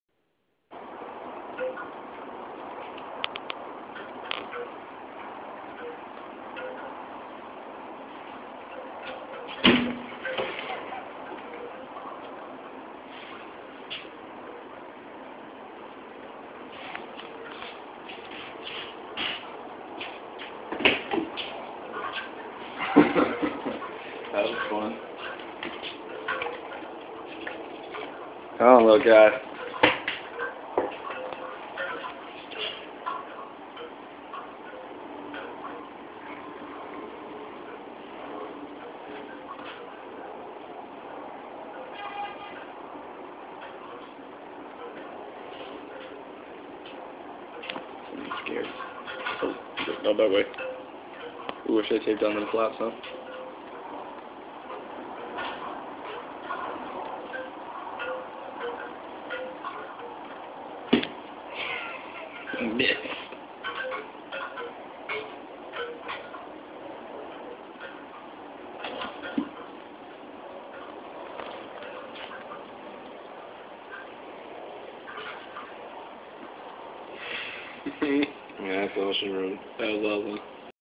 Arbiter Eats
Aztek Red Tail Boa eating a small mouse